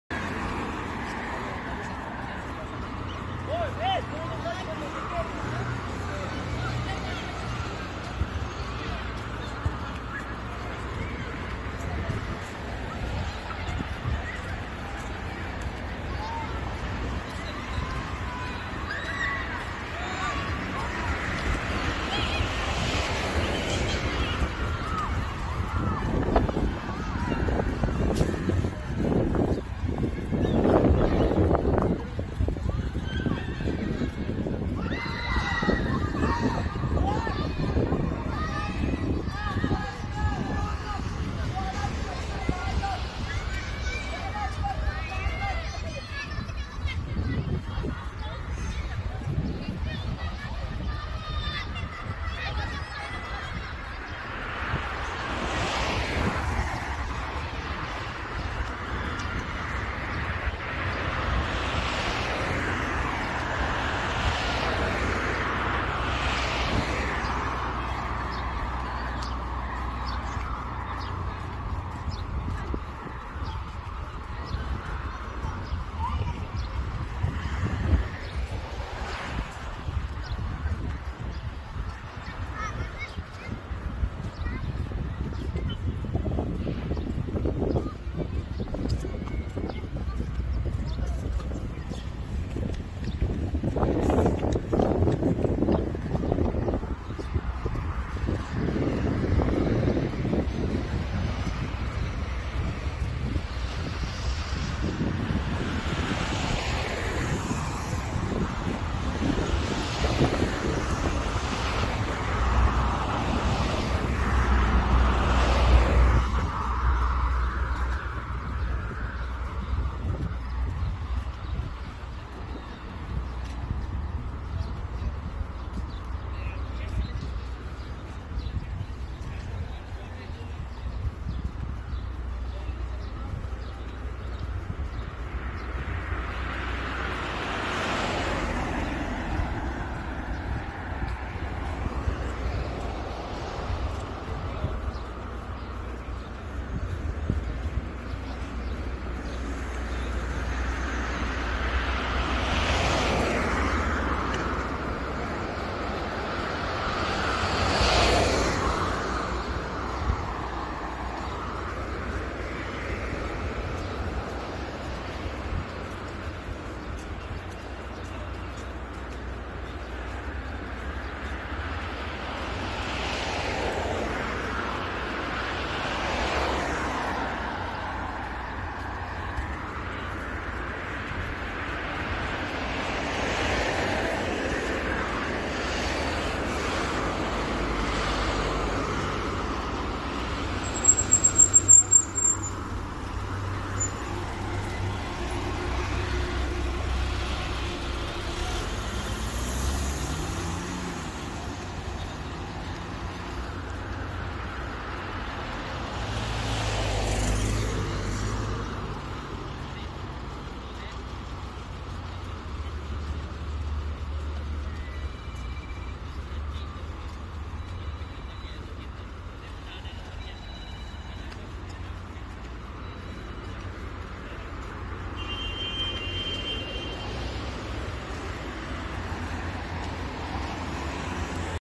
walking ‼️ around al haram makkah when pandemic corona
walking around the mecca when Covid-19 walk jalan

A quick walking video around the outside area of Masjid Al Haram, Makkah (Mecca), Kingdom of Saudi Arabia

shots with Samsung note 10.
taken after lockdown pandemi corona